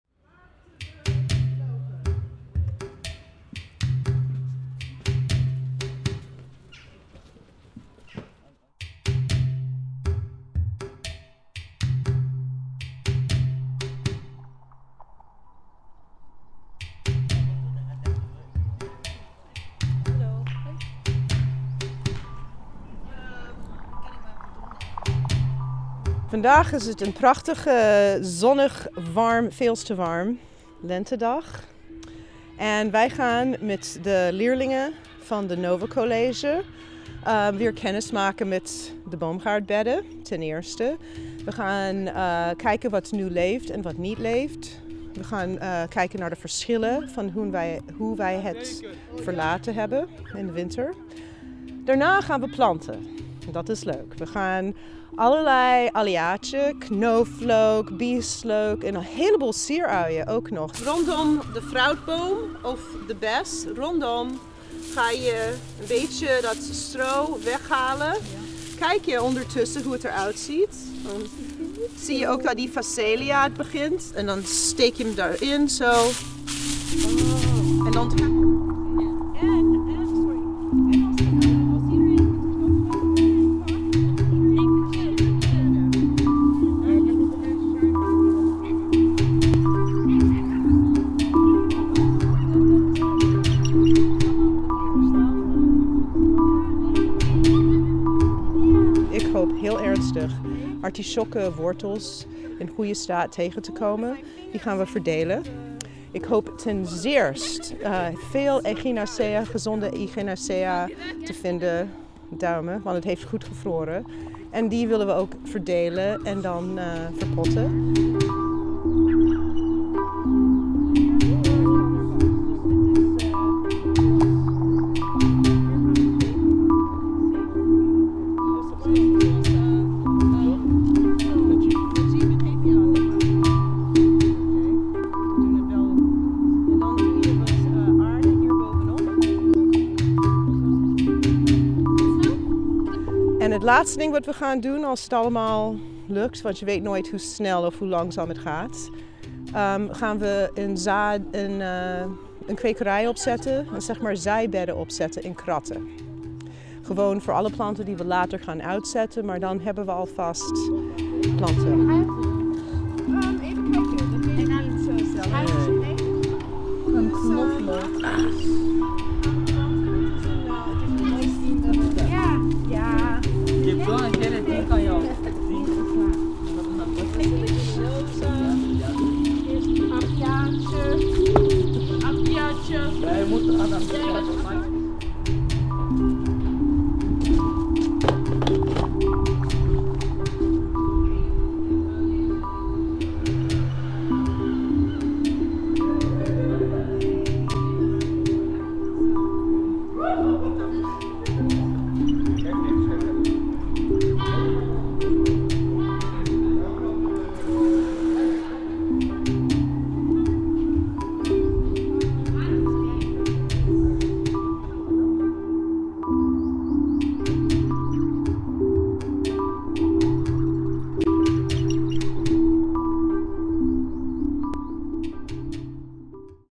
Foodscape Schilderswijk - Workshop
Foodscape Schilderswijk

Originally commissioned by Stroom Den Haag, Centre for Art and Architecture, URBANIAHOEVE is working on the development of a real edible city. Foodscape Schilderswijk is a design for a phased implementation of project locations that together describe an urban agriculture (UA) foodscape scenario within the context of Den Haag's Schilderswijk.

The project presupposes local commitment to knitting UA and adjacent infrastructures into a coherent infrastructure. Specifically, this means implementing a spatially contiguous planting scenario in which facilities, (educational) activities, and platforms for (food-related) cultural content are connected to a diversity of edible landscape architecture. When implemented, Foodscape Schilderswijk's components will connect and grow in the public space supported by locals and community initiatives. In September 2010 the project participants have planted more than 30 fruit trees, herbs, and berries in formerly inaccessible green spaces.

Documentation
Daniela Paes Leão
Merel Willemsen